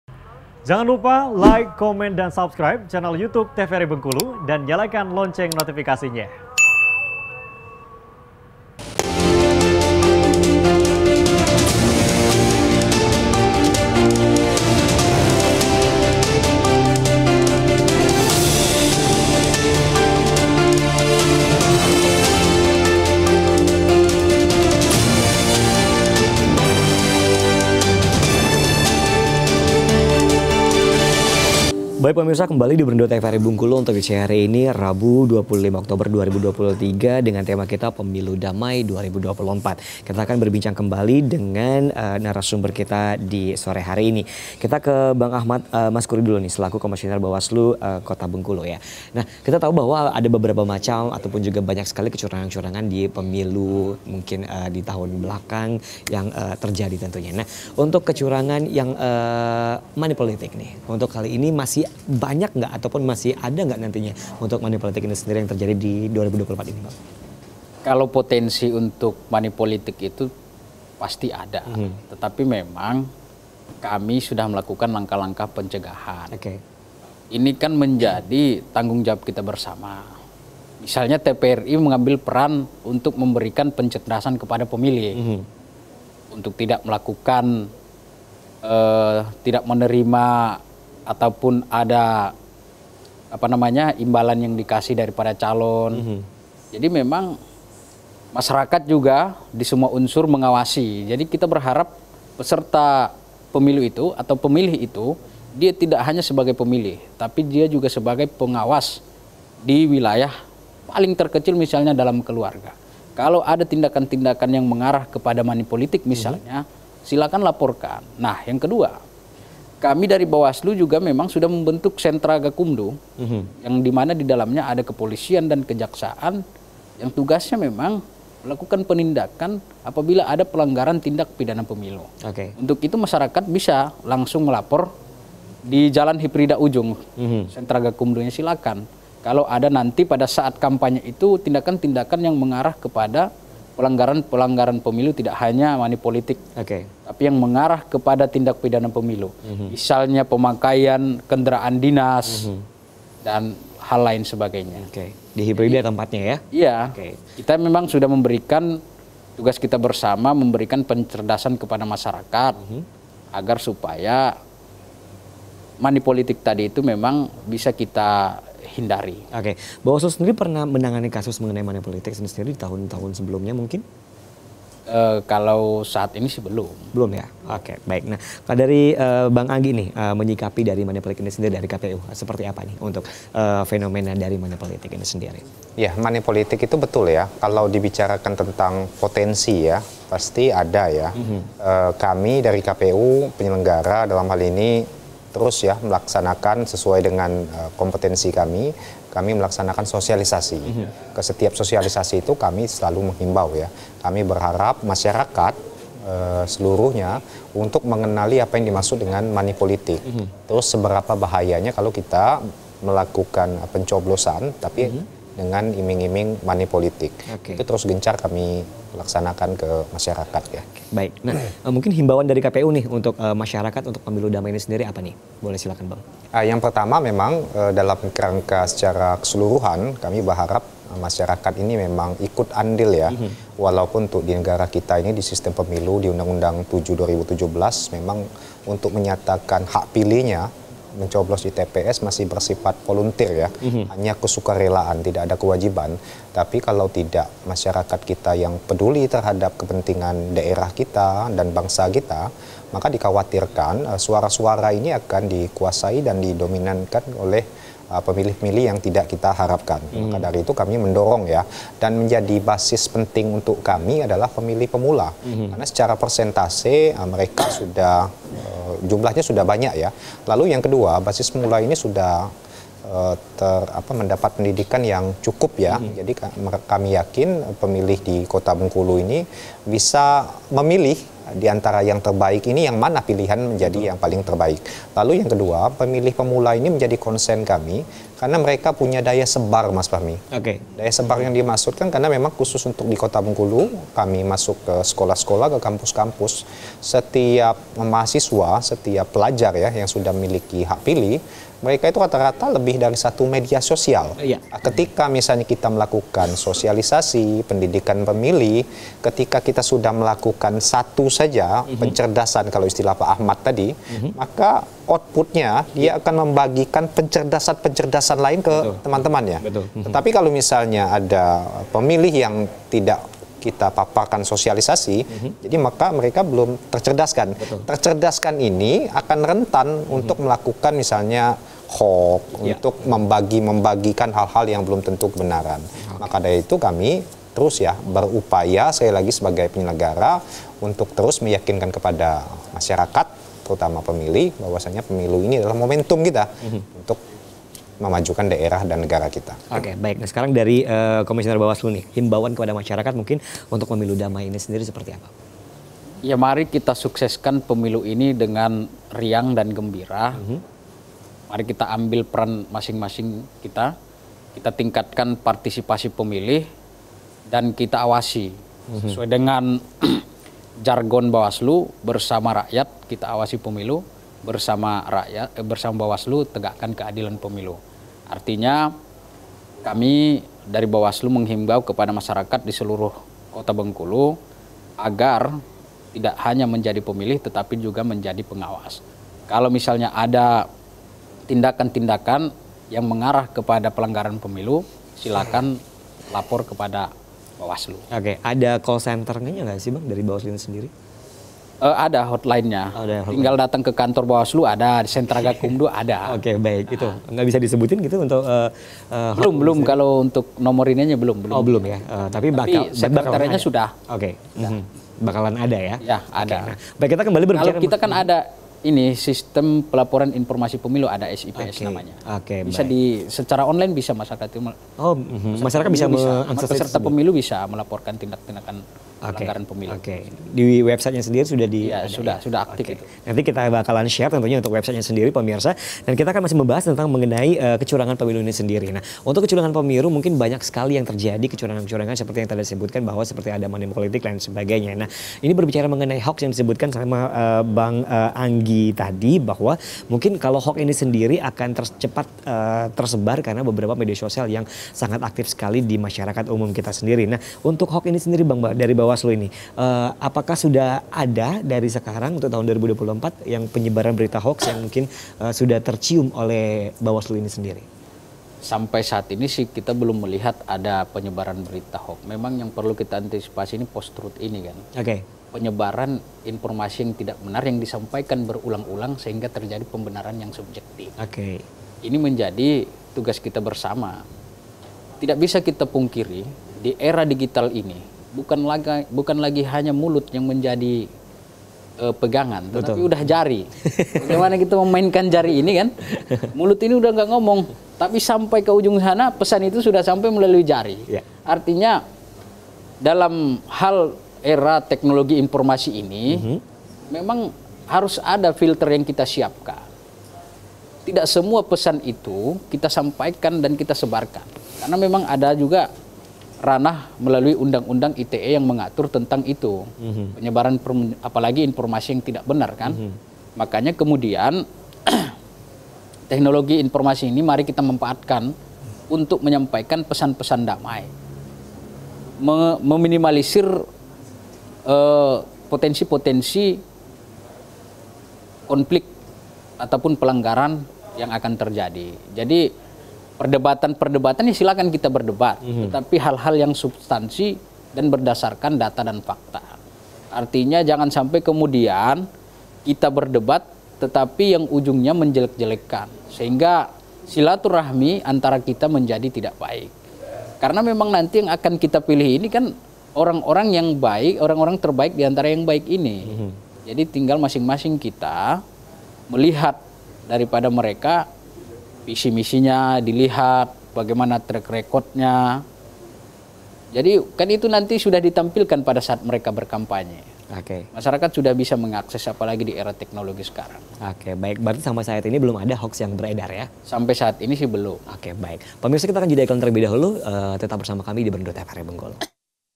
BERENDO PEMILU DAMAI SEGMEN 4
NS : ANGGI STEPHENSENT - KOMISIONER KPU KOTA BENGKULU
   : AHMAD MASKURI - KOMISIONER BAWASLU KOTA BENGKULU
MOD: KHAIRU FAHMI